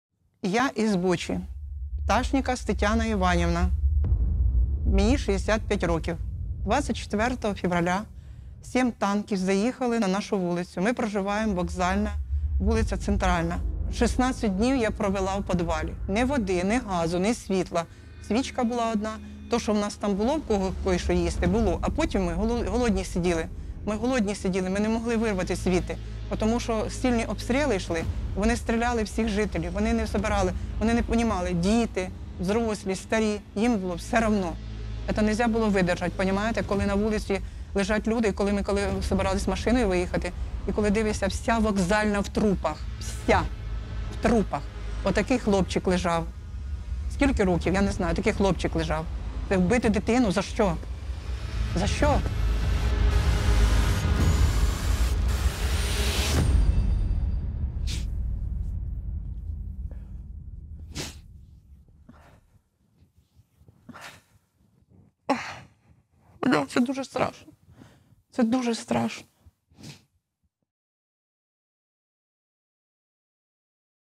StopRussiaNOW 🛑 - Tetiana's story
Her name is Tetiana Ivanivna Ptashnikas and she's from Bucha.
Hear the story about massacre in her hometown and help us StopRussiaNOW🛑